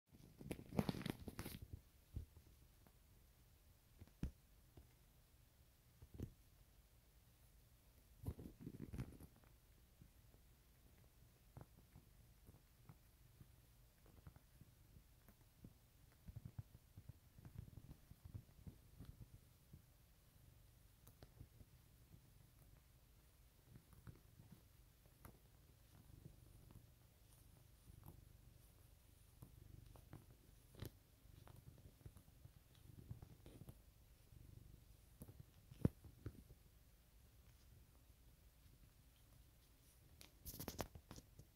I KNEAD you, momma!
Felix kneading my belly. Keep at it buddy. Flatten it out 😂